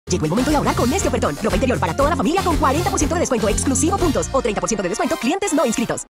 40% dto. o 30% dto. en ropa interior para toda la familia.
¡Ahorra con este ofertón! Aprovecha 40% dto o 30% dto. en ropa interior para toda la familia.  Válido solo el 21 Agosto de 2021.
 5.000 unidades disponibles.
*Aplican condiciones y restricciones.